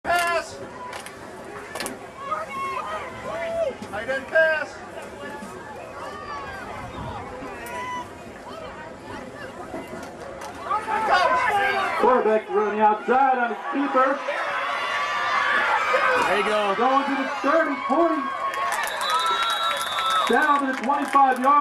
TYF A Team Gold vs Ellet 9-22-07
Andrew S runs the boot for a huge first down